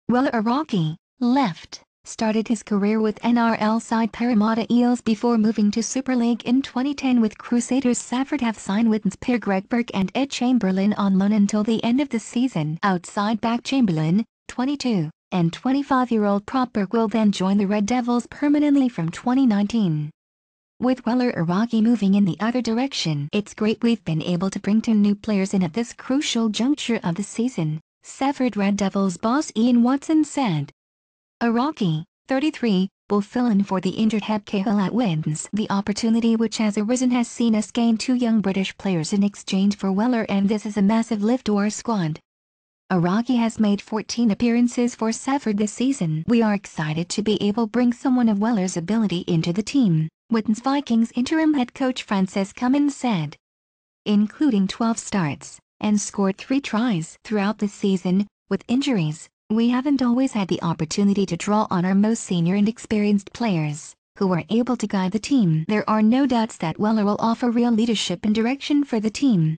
[2018 NEWS]Salford swap Weller Hauraki for Widnes' Ed Chamberlain and Greg Burke
Salford swap Weller Hauraki for Widnes' Ed Chamberlain and Greg Burke